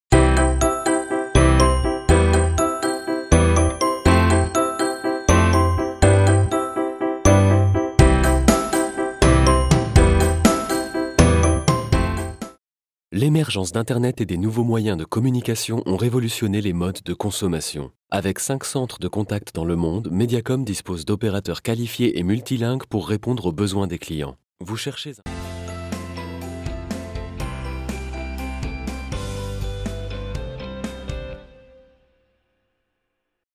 French European Voice Over Talent - FREUM08 Alain
Listen to a male French European Voice Over Talent. Choose FREUM08 Alain for your project.
More than 1000 active professional voice-over talents across the globe in any language. French European Voice Overs.